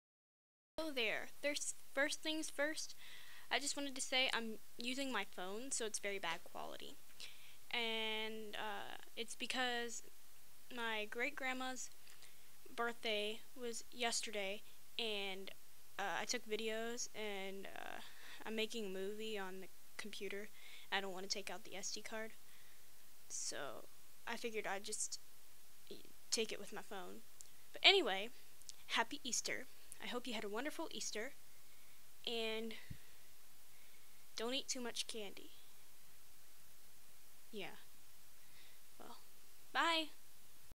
Happy Easter!
I'm taking this video with my phone so it is not good quality. I explain why I am using my phone in the video. just wanted to say have a Happy Easter!